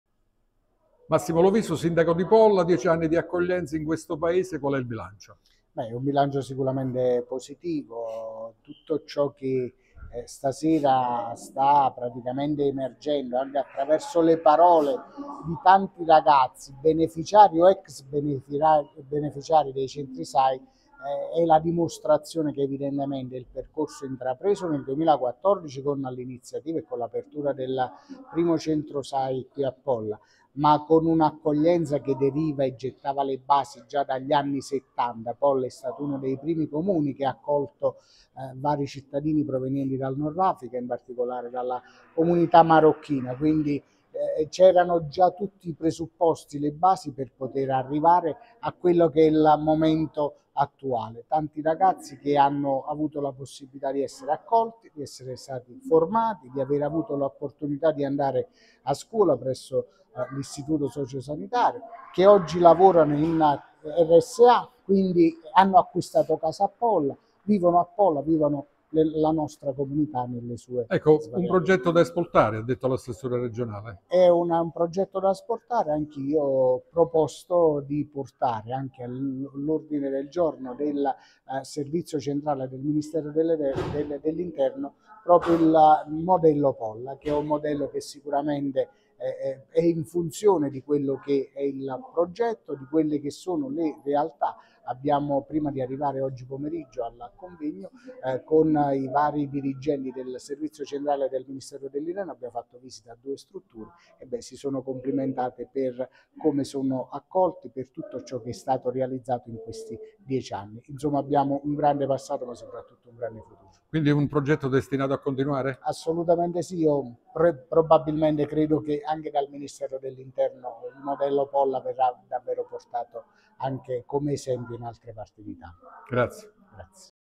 Massimo Loviso intervistato da Giuseppe d'Amico per i 10 anni del Sistema Accoglienza e Integrazione
Massimo Loviso, sindaco di Polla (SA), intervistato da Giuseppe "Geppino" d'Amico per @vallopiu7713  in occasione del decennale del S.A.I., Sistema Accoglienza e Integrazione a Polla (SA)